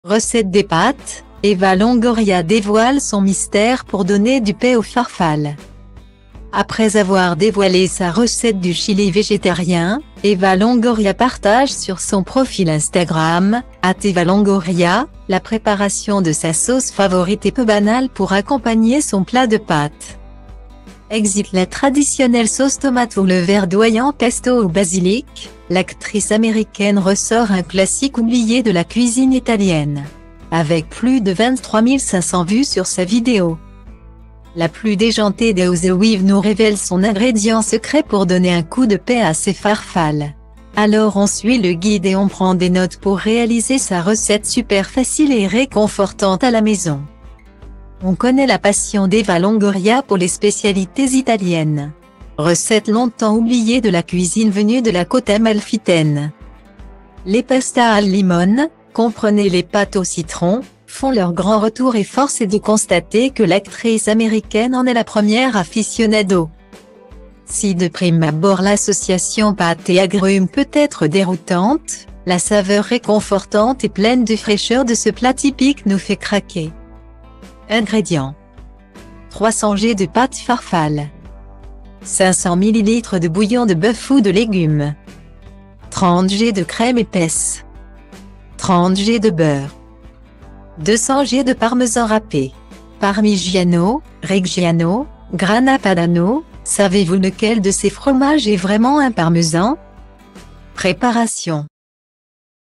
Recette des pâtes : Eva Longoria dévoile son mystère pour donner du peps aux farfalles
Après avoir dévoilé sa recette du chili végétarien, Eva Longoria partage sur son profil Instagram (@evalongoria) la préparation de sa sauce favorite et peu banale pour accompagner son plat de pâtes.Exit la traditionnelle sauce tomate ou le verdoyant pesto au basilic, l'actrice américaine ressort un classique oublié de la cuisine italienne ! Avec plus de 23 500 vues sur sa vidéo,la plus déjantée des housewives nous révèle son ingrédient secret pour donner un coup de peps à ses farfalles ! Alors on suit le guide et on prend des notes pour réaliser sa recette super facile et réconfortante à la maison.